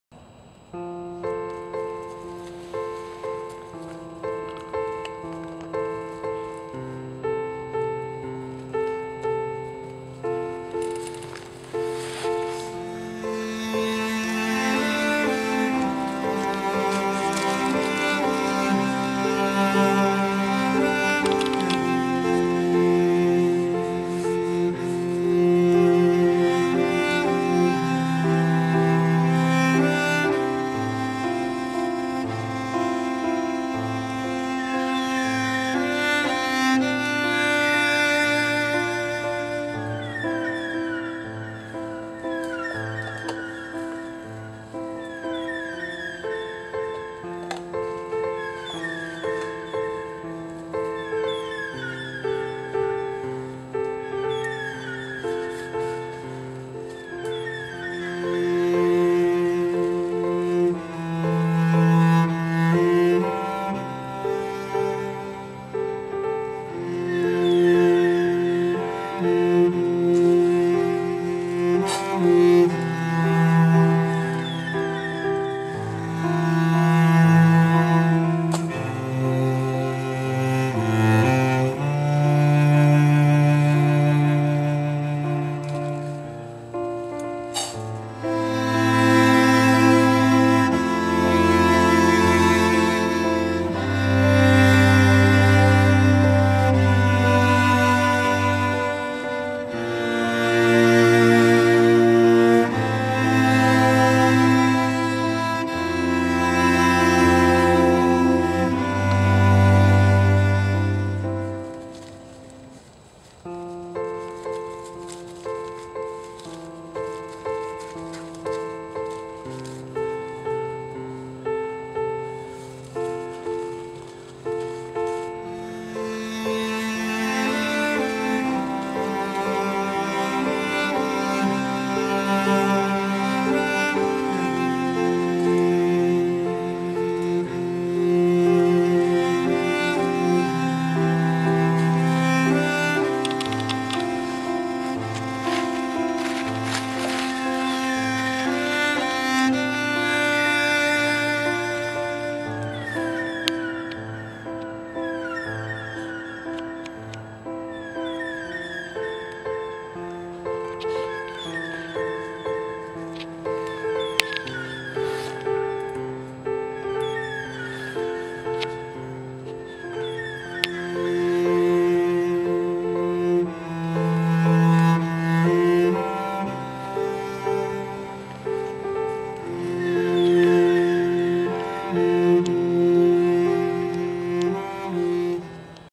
Raccoon family comes for a musical breakfast
This is the first soundtrack from Greedyman Productions.  A raccoon family come for breakfast and enjoys a couple of donut holes